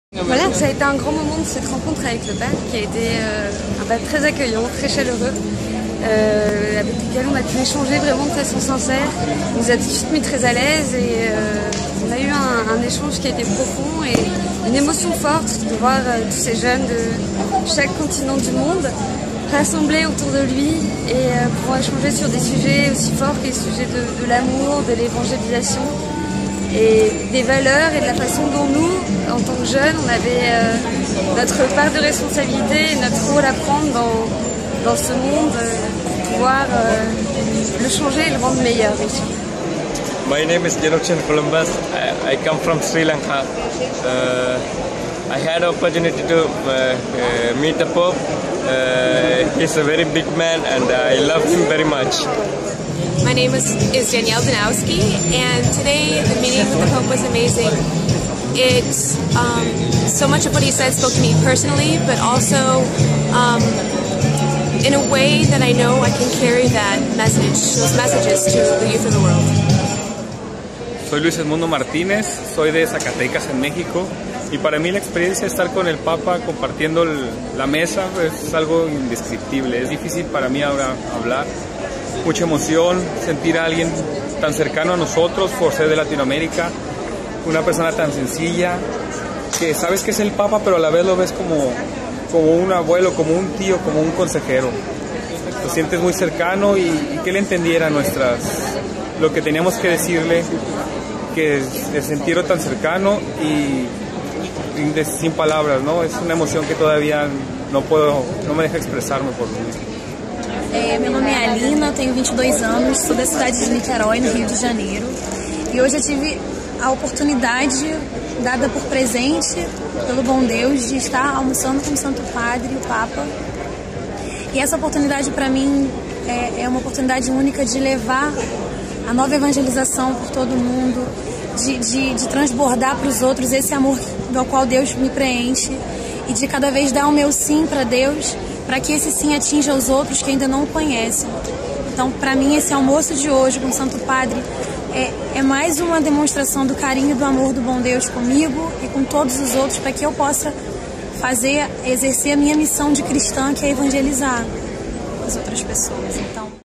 Alcuni dei giovani che hanno partecipato al pranzo con Papa Francesco a Rio de Janeiro
Emozionati parlano di "un'esperienza che non dimenticheranno mai", di un "regalo", di "un'opportunità speciale", di un'emozione che ora vogliono condividere con gli altri giovani della Gmg. Si sono presentati così alla stampa i 12 giovani che hanno pranzato oggi con Papa Francesco al palazzo arcivescovile. Era presente anche l'arcivescovo di Rio de Janeiro, mons. Orani João Tempesta. Un pranzo lungo, durato più di un'ora e mezza, dalle 12.35 alle 14.15, e si è "mangiato di tutto", raccontano i ragazzi. Scelti per sorteggio i giovani rappresentavano tutto il globo: 2 per ogni continente, più 2 rappresentanti per diritto del Brasile, il Paese ospitante della Gmg. Per l'Oceania: Nuova Zelanda e Australia; per l'Europa: Portogallo e Francia; per l'America del Nord: Messico e Usa; per l'America del Sud: Argentina e Colombia; per l'Asia: Sri Lanka e Russia